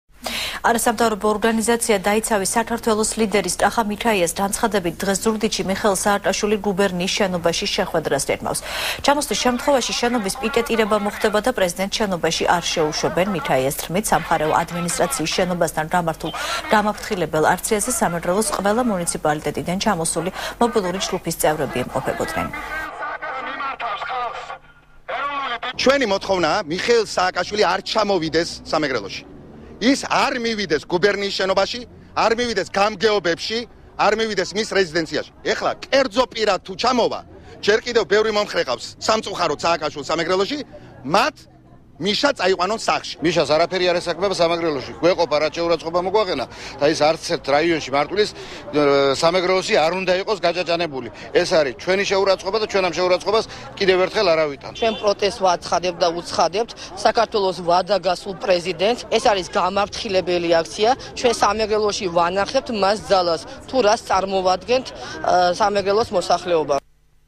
ახალი 3 | გამაფრთხილებელი აქცია | 08.02.13
არასამთავრობო ორგანიზაცია „დაიცავი საქართველოს" ლიდერის, კახა მიქაიას განცხადებით, დღეს ზუგდიდში მიხეილ სააკაშვილი გუბერნიის შენობაში შეხვედრას გეგმავს. ჩამოსვლის შემთხვევაში შენობის პიკეტირება მოხდება და პრეზიდენტს შენობაში არ შეუშვებენ. მიქაიას თქმით,  სამხარეო ადმინისტრციის შენობაშთან გამართულ გამაფრთხილებელ აქციაზე სამეგრელოს ყველა მუნიციპალიტეტიდან ჩამოსული მობილური ჯგუფის წევრები იმყოფებოდნენ.